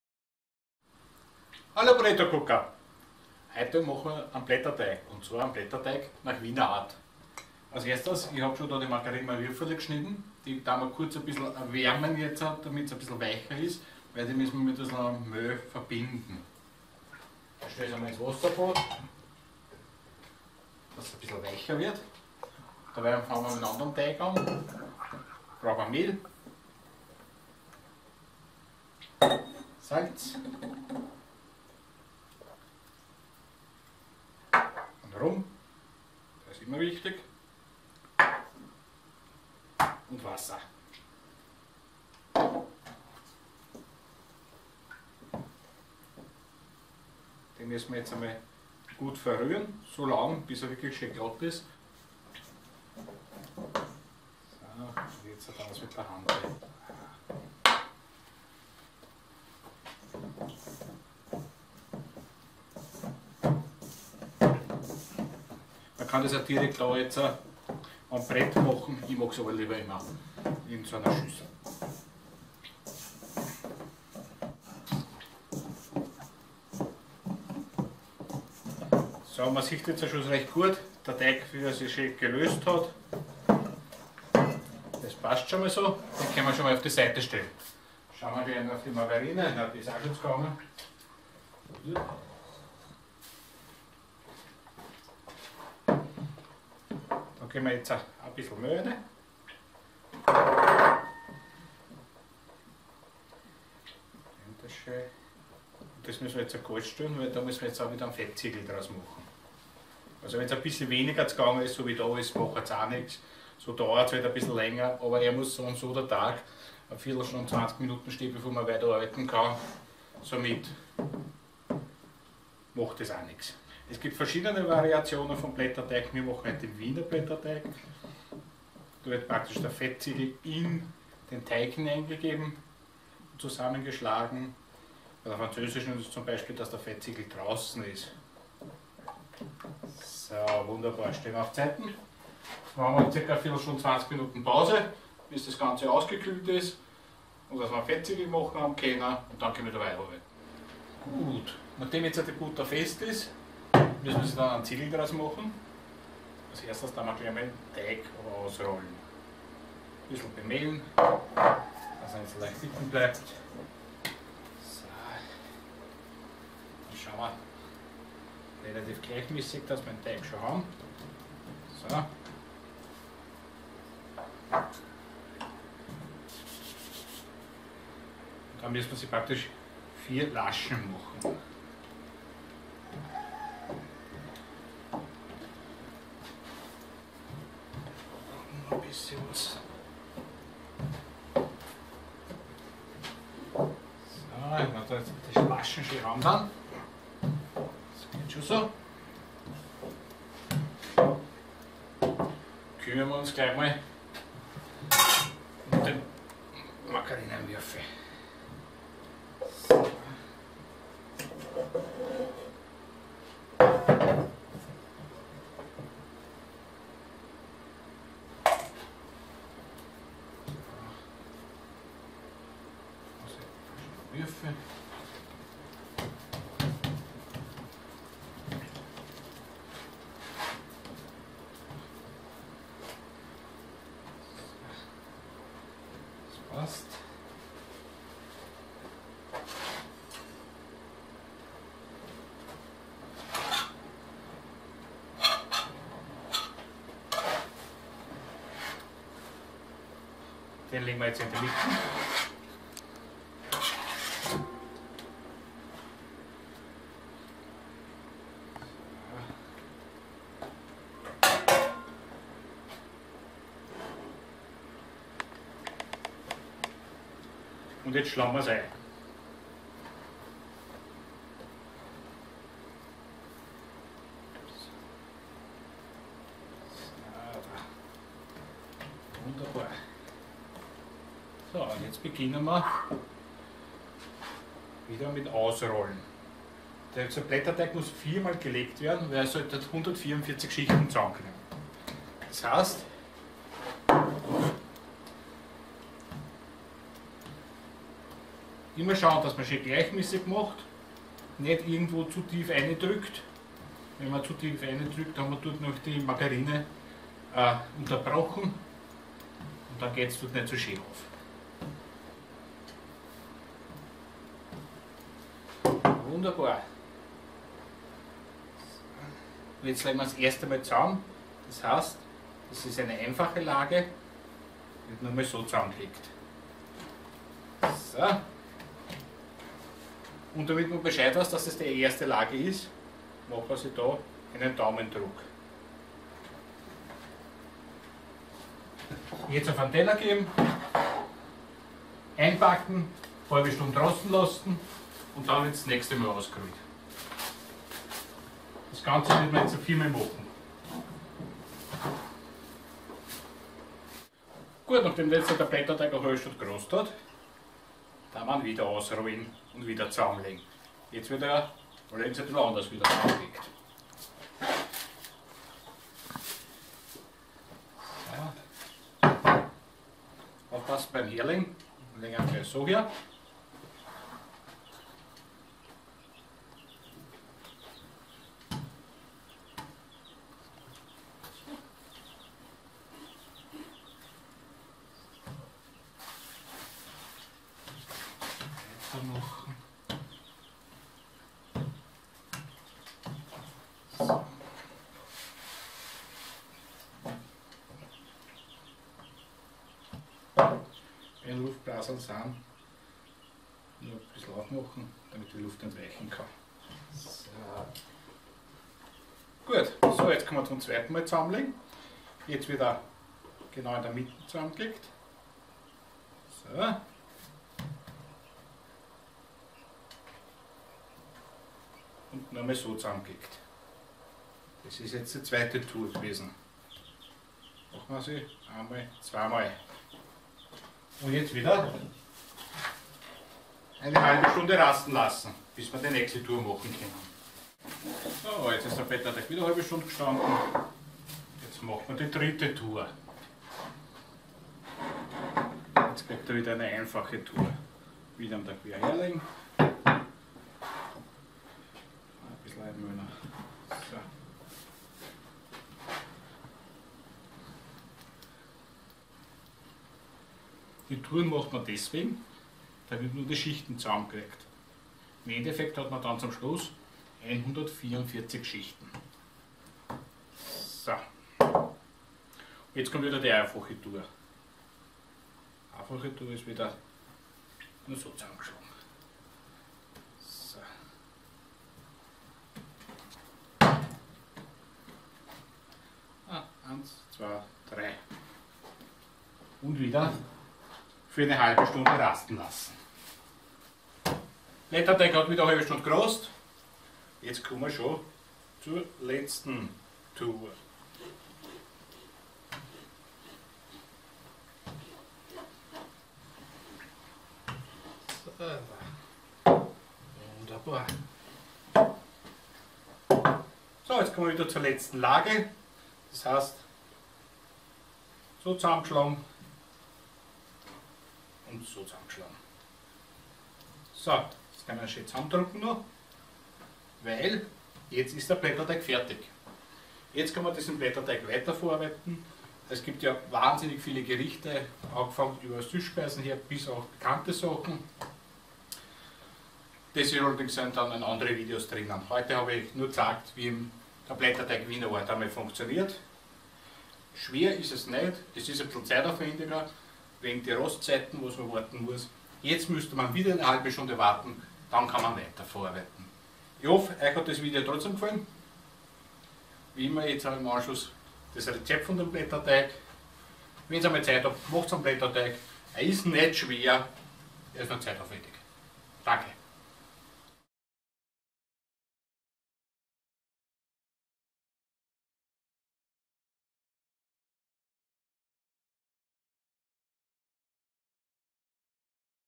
Blätterteig Wiener Art
Rezept
Teig
600g glattes Mehl Typ 700
320g Wasser
7g Salz
15g Rum
Fettziegel
500g Ziehmargarine
50g Mehl glatt Type 700